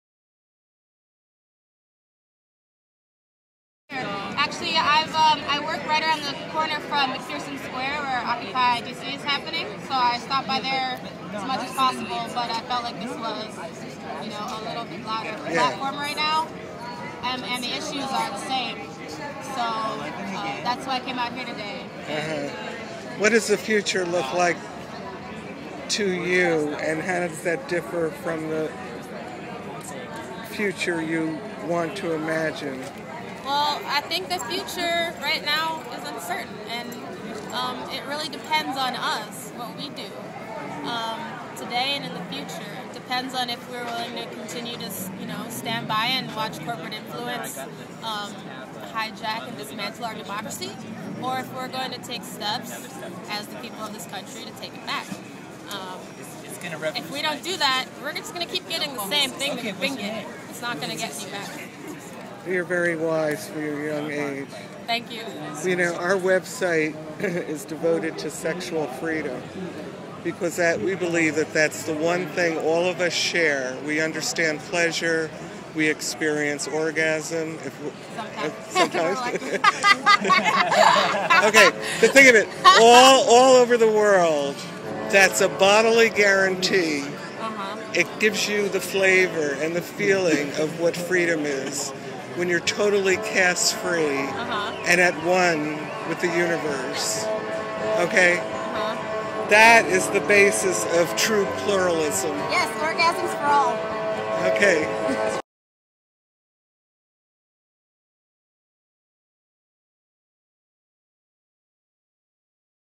The Sexual Freedom Project: The Future Depends On Us
As 2011 draws to a close, it is a natural time to pause and reflect on the year that is ending, and look forward to the new year that is opening up before us. What sort of future do you envision, next year and beyond? Do you think things will get better in terms of our politics, our economy, our sexual freedoms? What do you want to change about the future? What role will the Occupy movement, taking place across the country, play in making a better future? How are you personally going about effecting the kinds of changes that you hope to see?
Let us know what you think. Make a video, write a poem, song, or an essay — or even create an original work of art — and express your thoughts on these topics. we will send you a free t-shirt to thank you.